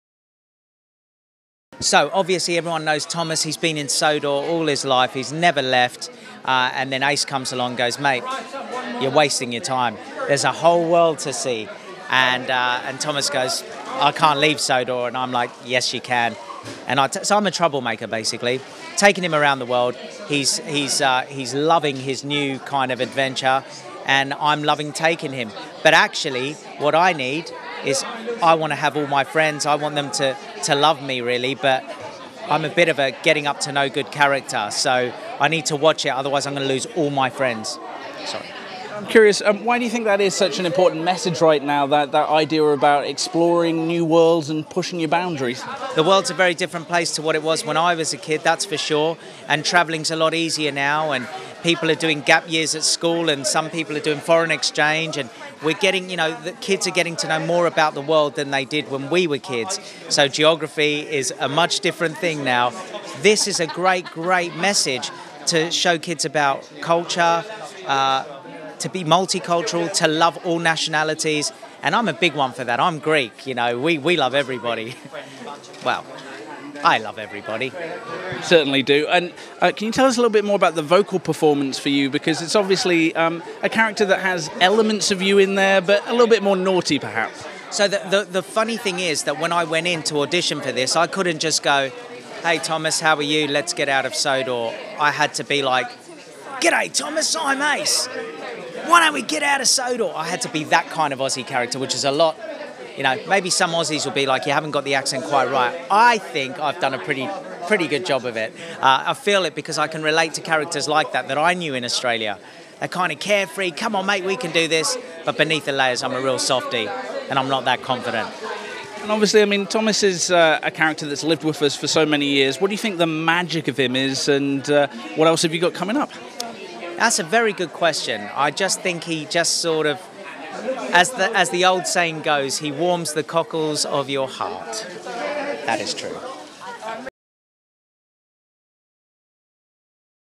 Peter Andre Interview Thomas & Friends Big World! Big Adventures! Premiere
Red Carpet News TV talks to cast and VIP guests at the London premiere screening for new children’s film Thomas and Friends Big World! Big Adventures! The Movie. Interviews include Peter Andre, John Hasler, Yvonne Grundy, Ashley Walters and Milkshake presenter Nathan Connor.